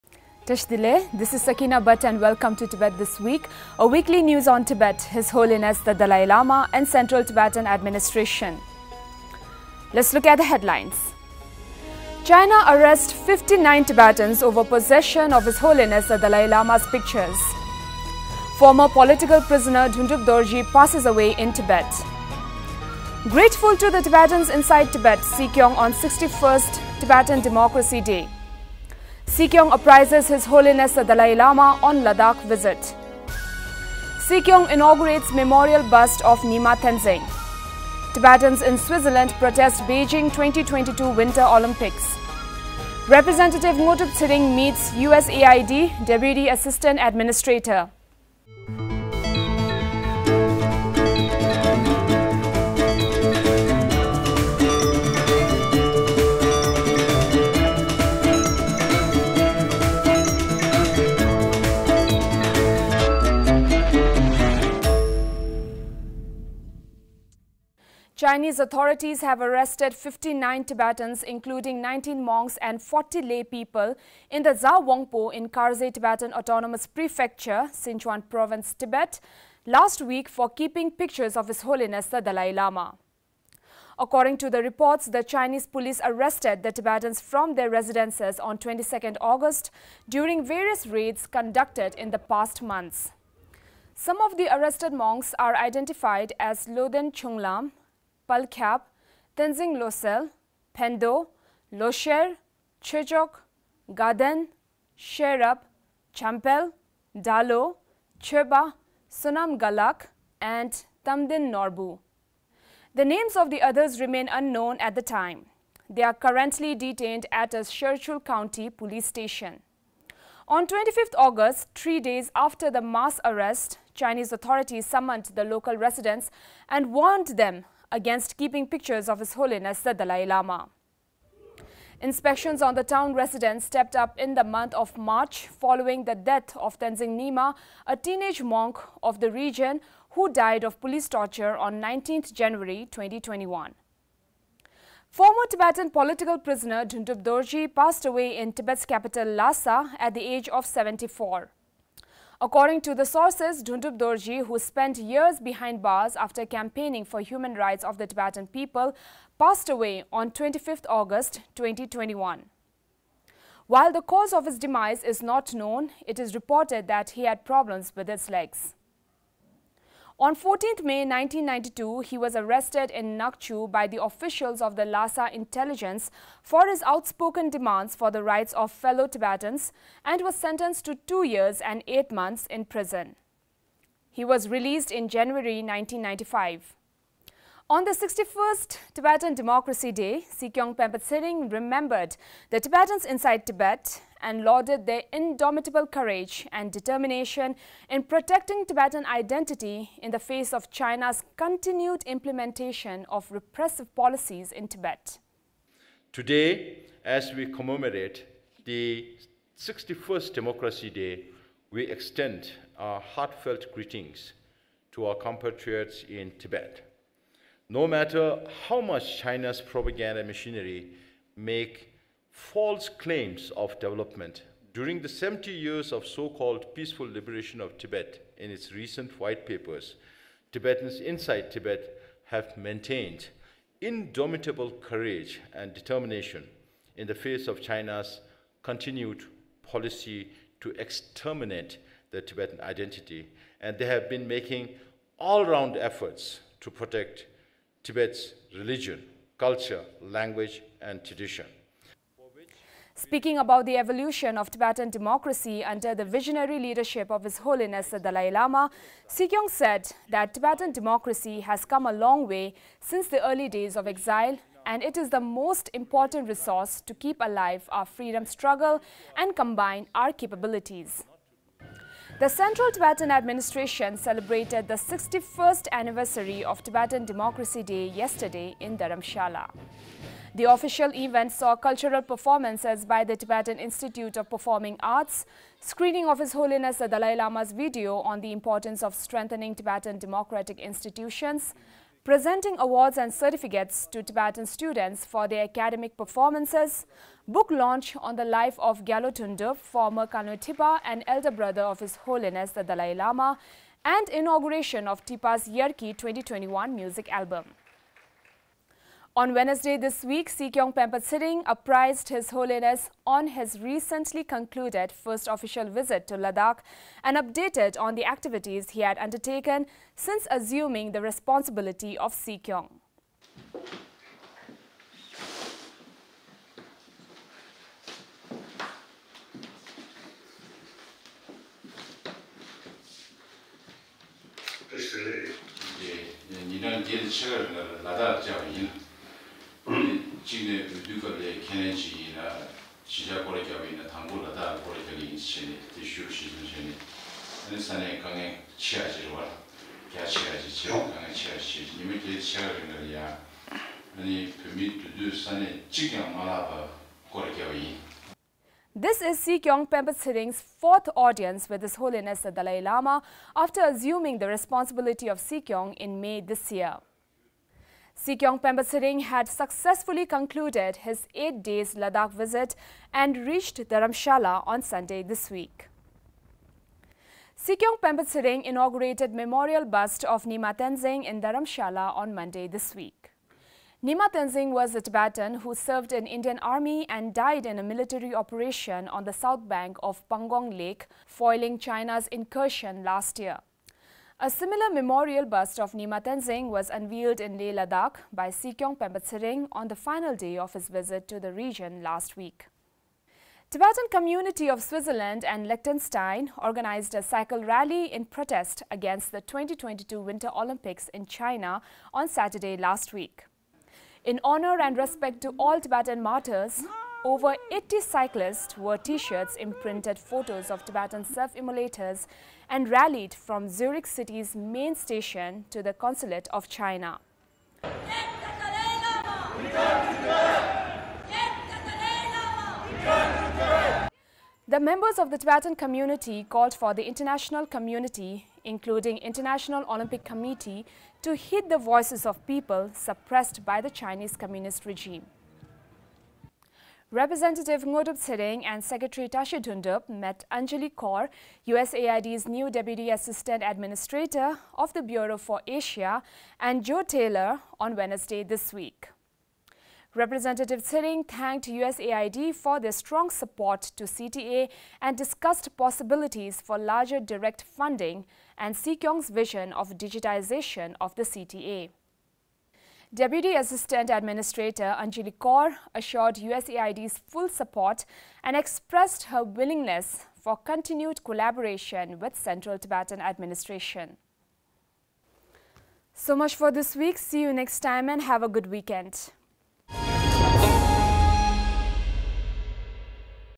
Tibet This Week - 03 September, 2021
1. China Arrests 59 Tibetans over Possession of His Holiness the Dalai Lama’s Pictures
2. Former Political Prisoner Dhondup Dorjee Passes Away in Tibet
3. ‘Grateful to the Tibetans Inside Tibet’- Sikyong on 61st Democracy Day
4. Sikyong Apprises His Holiness the Dalai Lama on Ladakh Visit
5. Sikyong Inaugurates Memorial Bust of Nyima Tenzing
6. Tibetans in Switzerland Protest Beijing 2022 Winter Olympics
7. Representative Ngodup Tsering Meets USAID Deputy Assistant Administrator